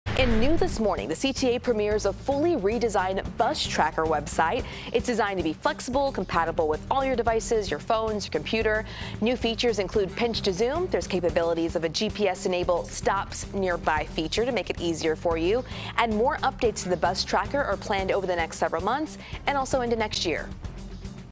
CTA premieres new bus tracker
It's designed to be flexible and compatible with all devices, from smartphones to desktops.